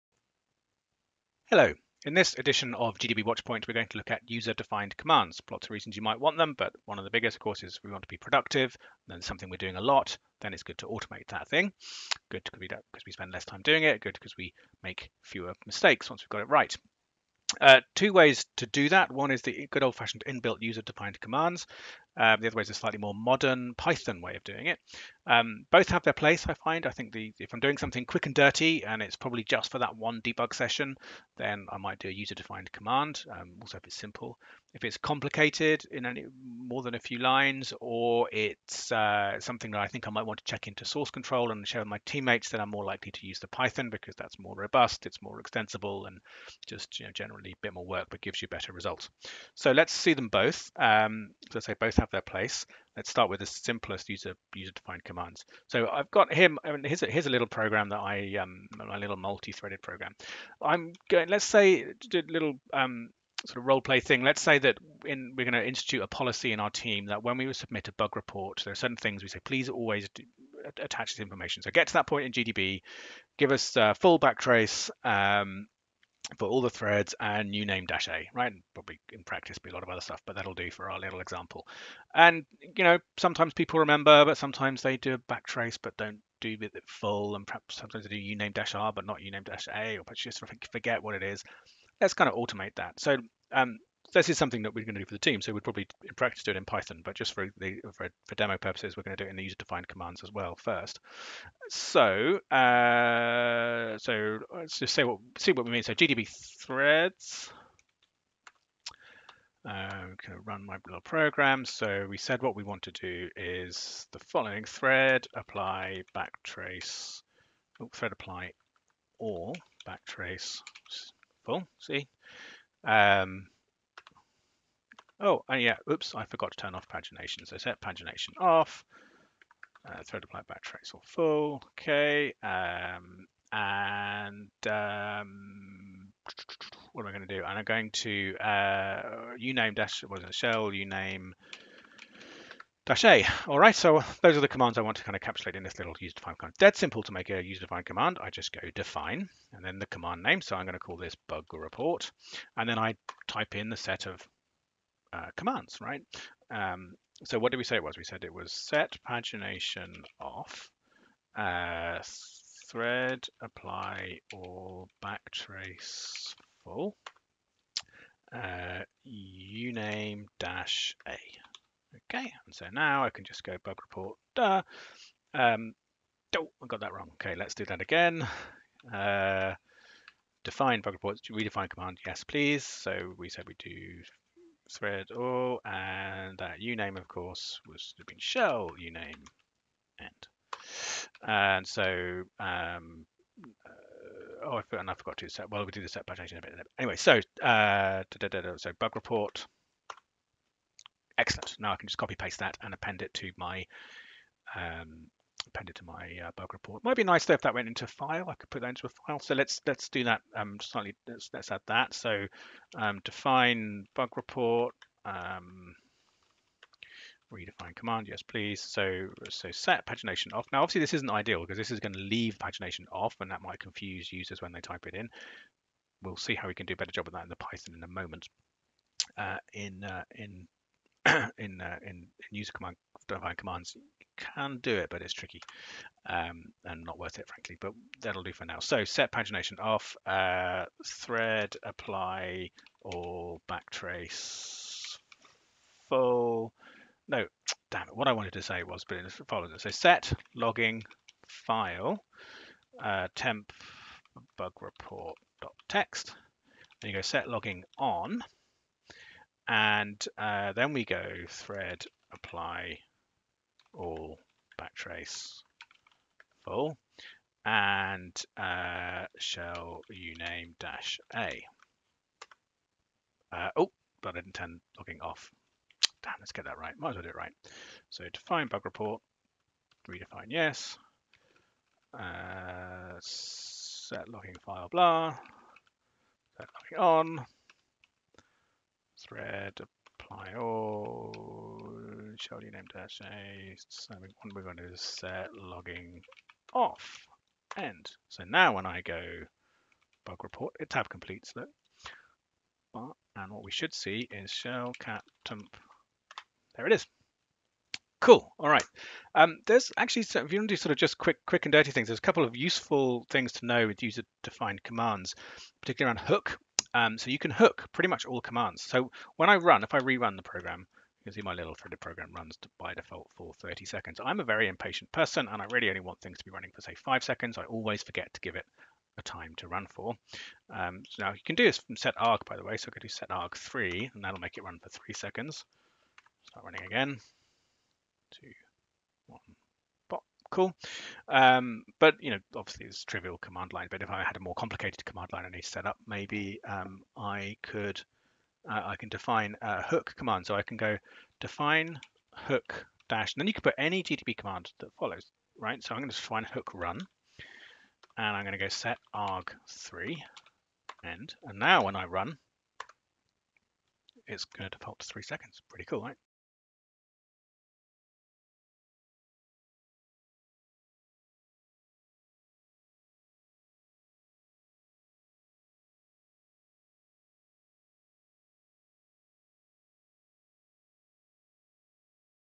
GDB User Defined Commands - Part 1
This is the first GDB tutorial in a series of three GDB articles exploring the various ways of getting the most out of user-defined commands in GDB. In this tutorial, Greg Law explores GDB’s built-in user-defined command option. Check it out.